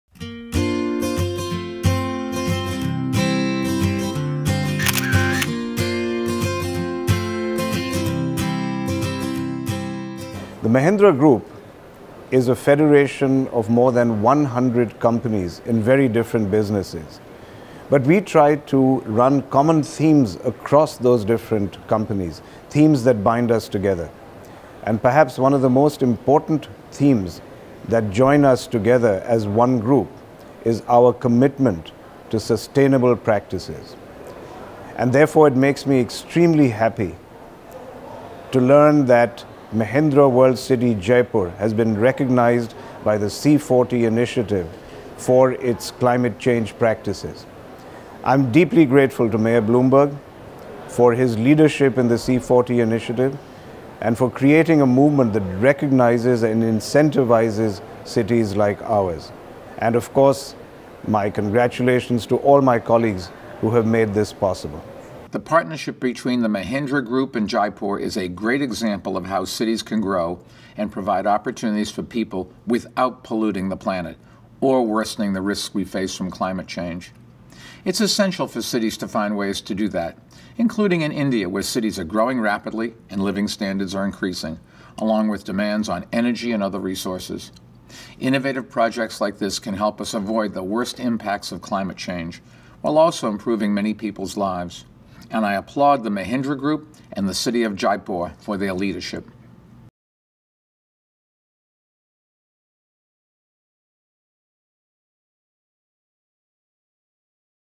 Mahindra World City Jaipur C40 Video
Mahindra World City Jaipur becomes Asia's first project to receive C40 Stage 2 Certification.